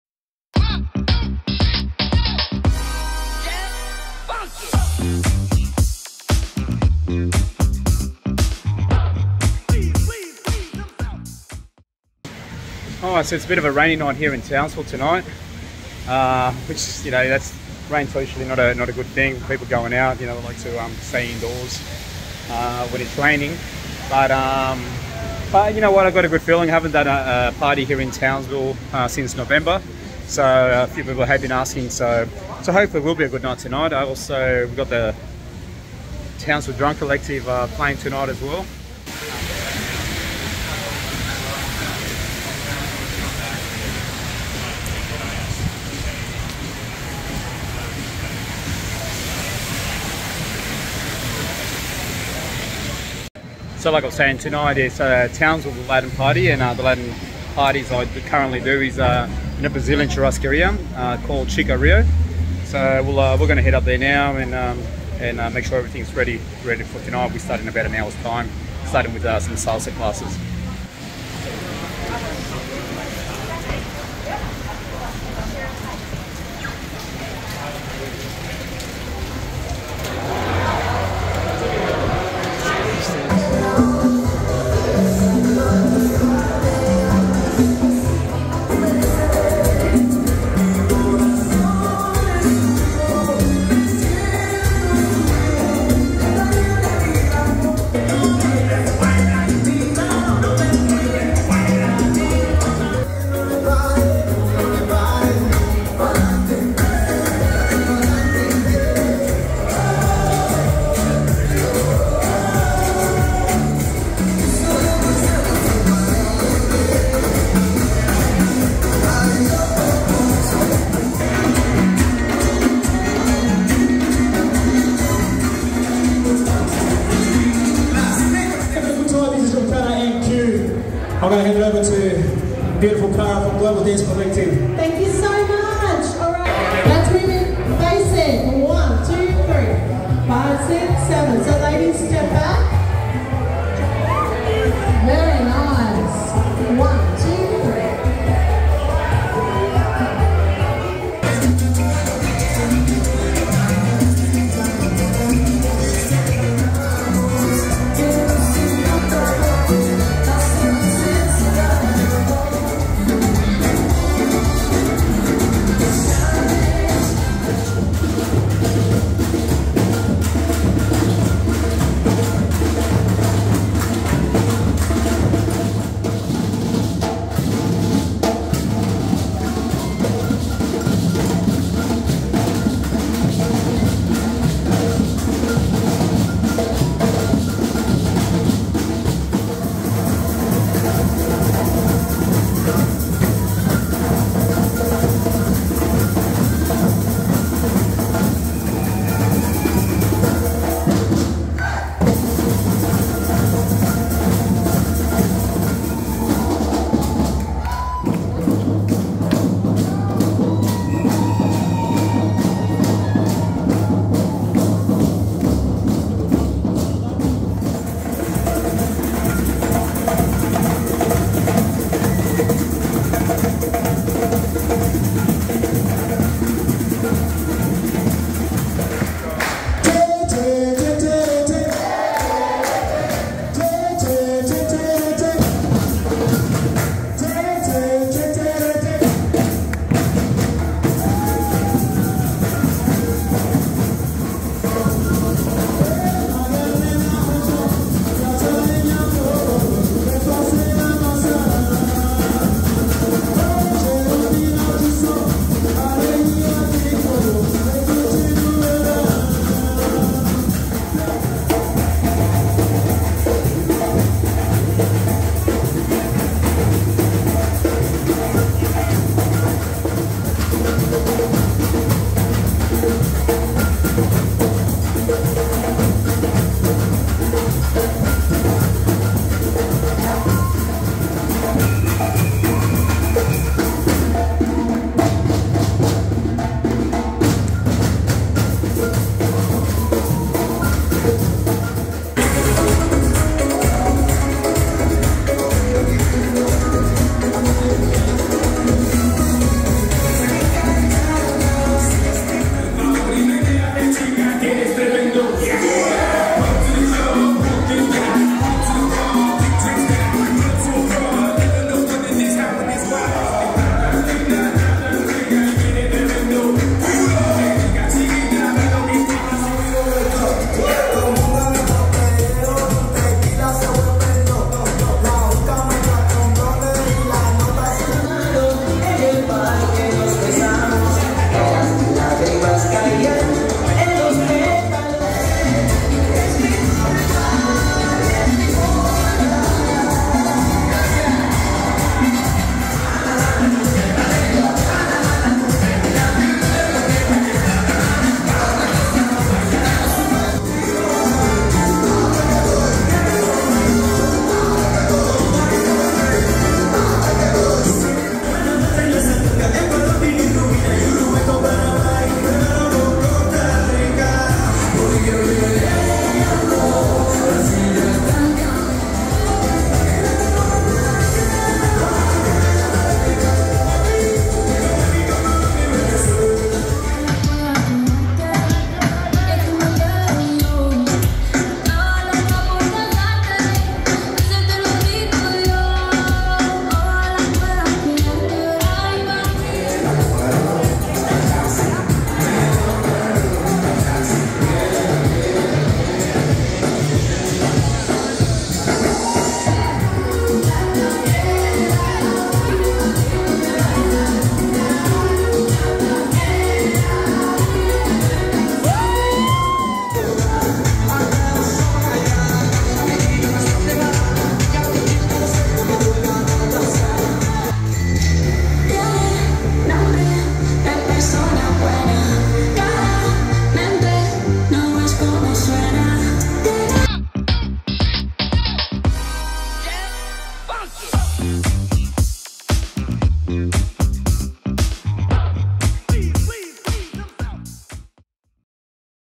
"Salsa, Drums, and Fun: My Night at Tropicana NQ in Townsville!"
Welcome to my latest gig blog! In this video, I'm excited to take you to Tropicana NQ in Townsville, where The Global Dance Collective is offering a free salsa class. You'll get to experience the vibrant energy of the dance floor and learn some new moves with expert instructors. But that's not all - the Townsville Drum Collective is also performing, adding their rhythmic beats to the party. It's a night full of music, dancing, and community, and I can't wait to share it with you. So, come join me at Tropicana NQ and let's dance the night away!